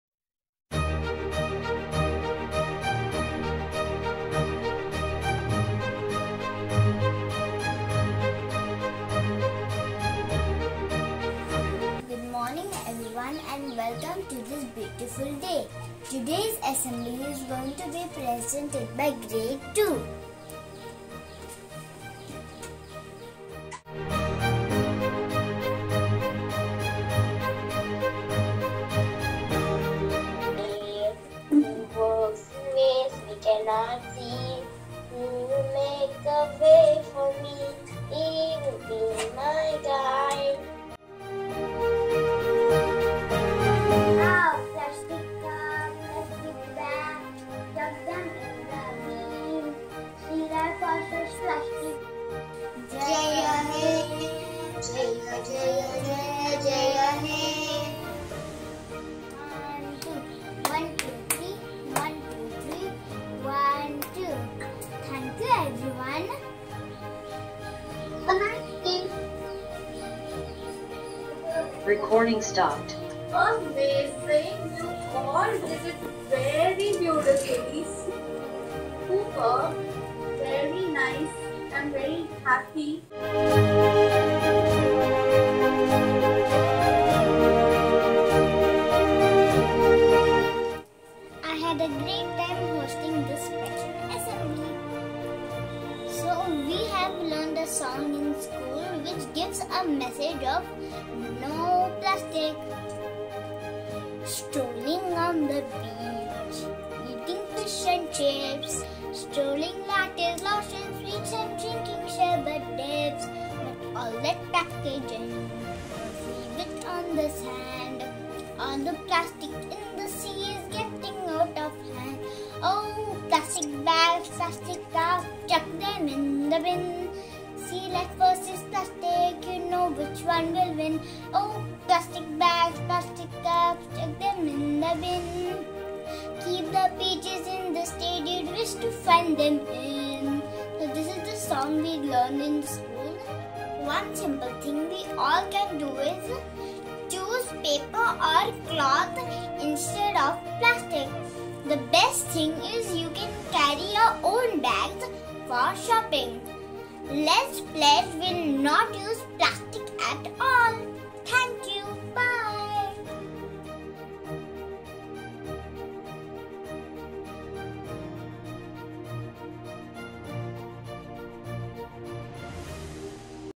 Svanik as a Host at School Event |Vlog18
Svanik was chosen to be the host at "Special Assembly July 2021" held at C. P. Goenka International School. Special Assembly is held twice a year with a specific theme. "Plastic Free" was the theme for this assembly. Do not miss the song and a message from Svanik towards the end of the video.